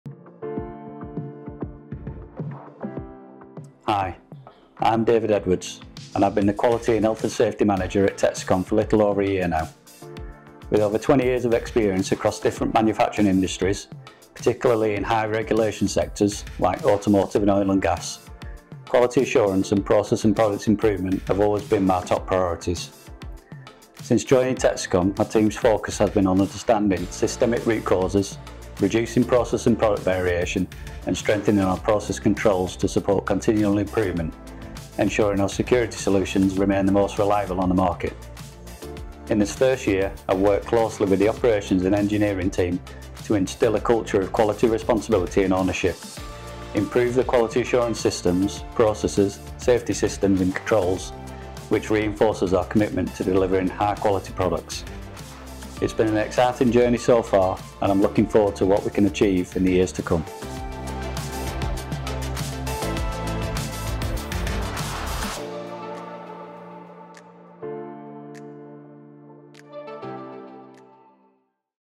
Meet David Edwards: Texecom's Quality, Health & Safety Manager
Join us as David Edwards reflects on his first year as Quality, Health & Safety Manager at Texecom. With over 20 years of experience in high-regulation industries like automotive and oil & gas, David brings a passion for quality, precision, and innovation. Learn how he's enhancing product standards, streamlining processes, and helping Texecom deliver industry-leading security solutions. Discover more about David's journey and Texecom's commitment to excellence!